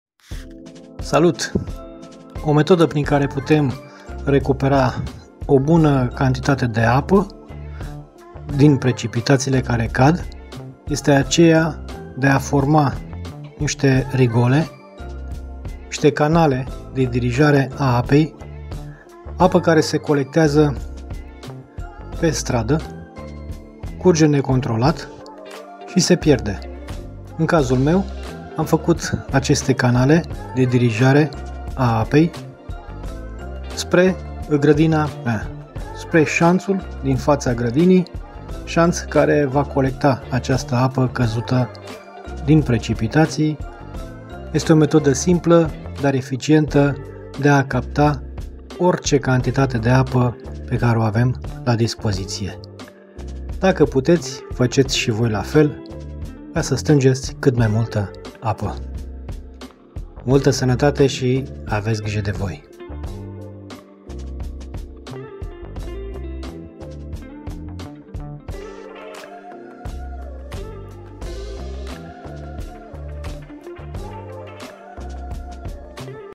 Colectez apă de ploaie de pe stradă.
Apa este o resursa vitala pentru agricultura. Orice sursa de apa ntrebuie folosita pentru plantele din gradina.